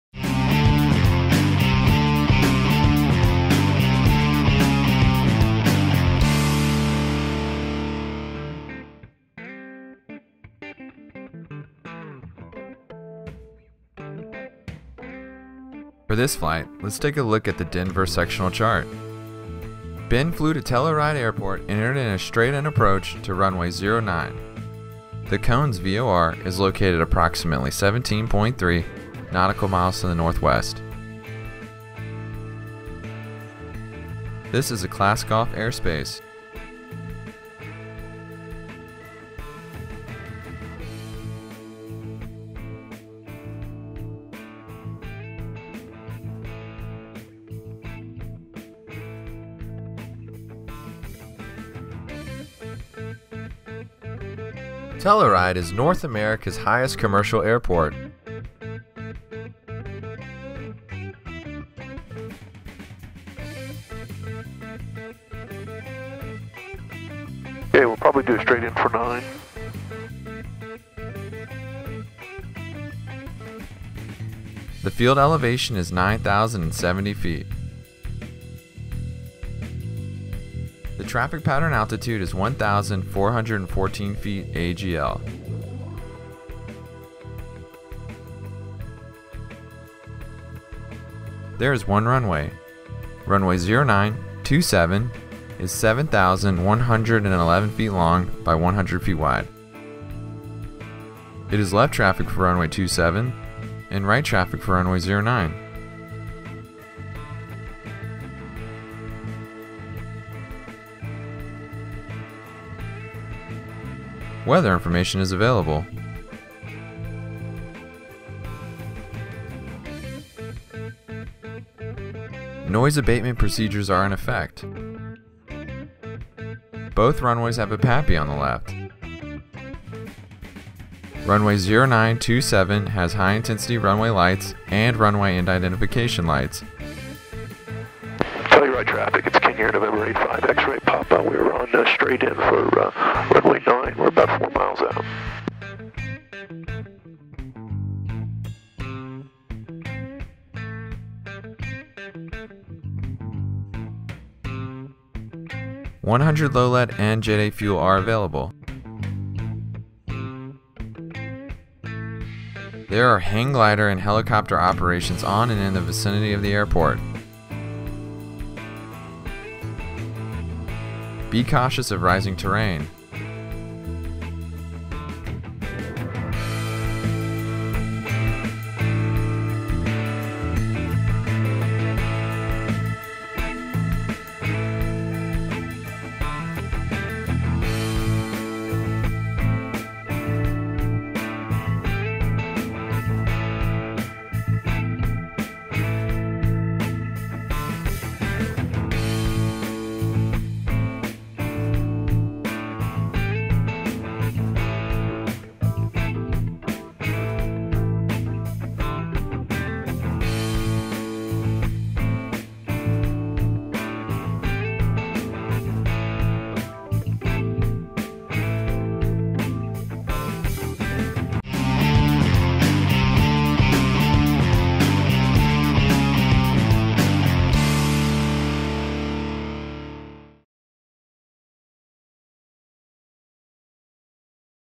Flying with Tony Arbini into the Telluride Regional Airport (KTEX)-Telluride, California
Landing patterns brings you another in-depth video, guiding you on our landing into the Telluride Regional Airport (KTEX). Our landing into the Telluride Regional Airport in Telluride California provides you with lots of information on charts, weights, altitudes, and airport information for pilots to plan for a safe landing!